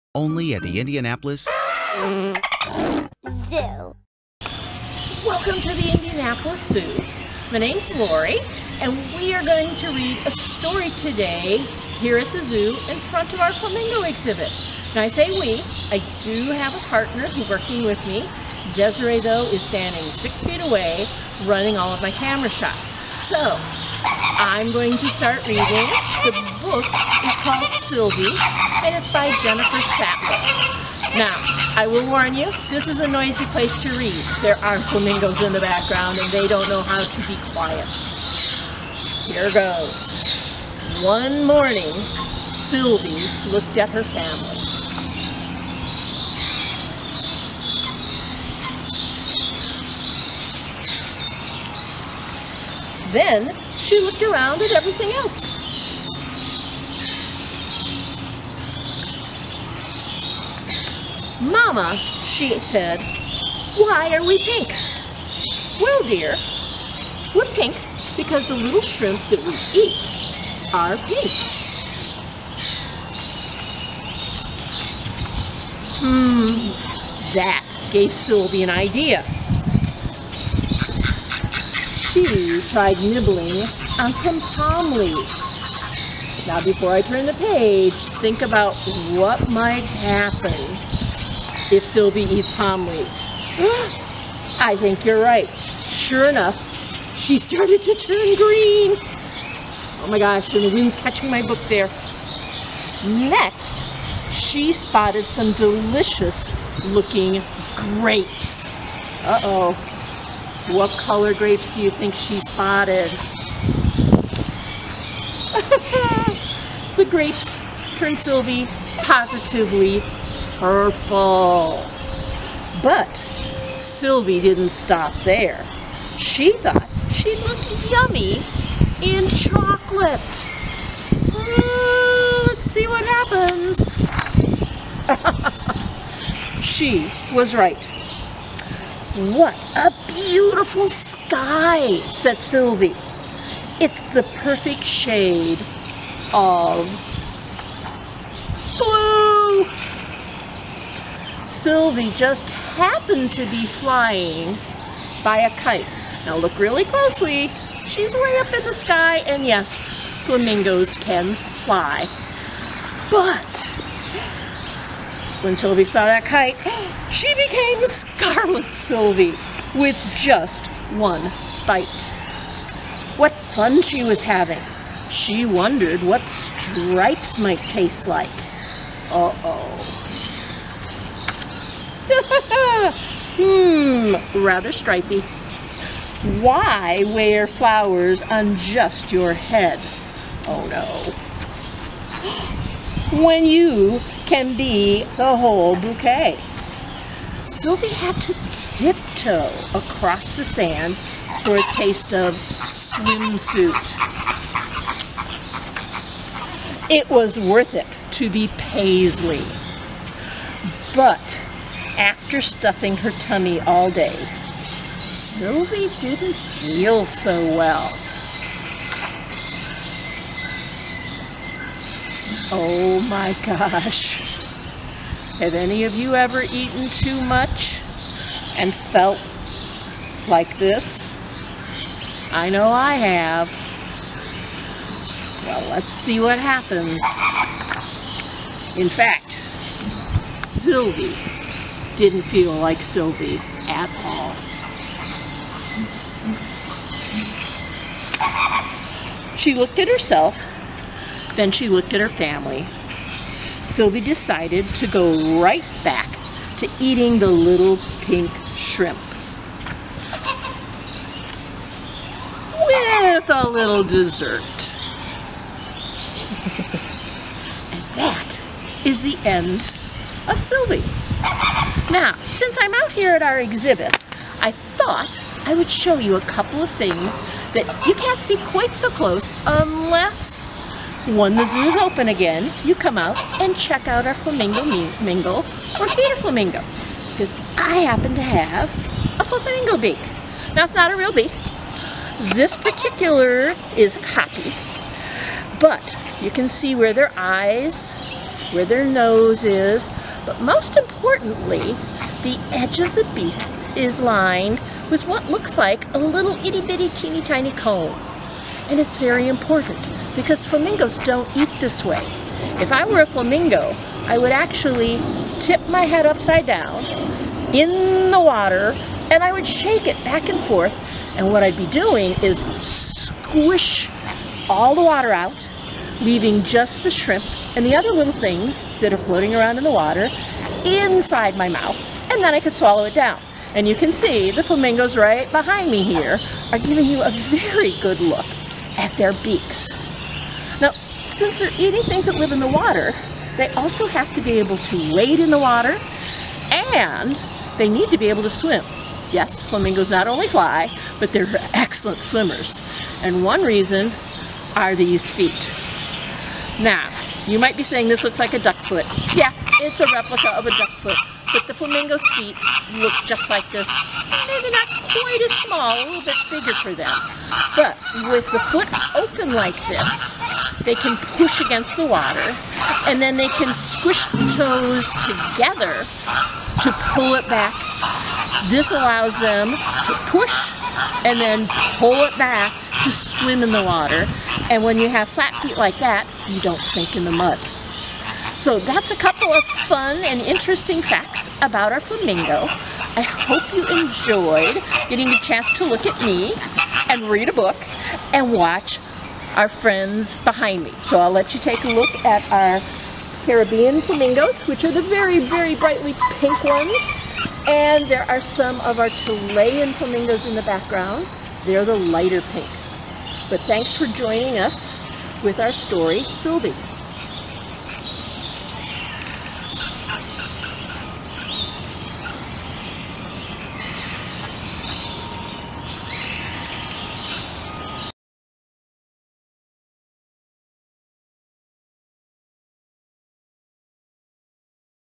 Indianapolis Zoo Reads: Sylvie The Colorful Flamingo
Indianapolis Zoo educator Laurie is here to share one of our favorite books, Sylvie: The Colorful Flamingo by Jennifer Sattler. Laurie is joined by our own very colorful birds, Chilean and Caribbean flamingos.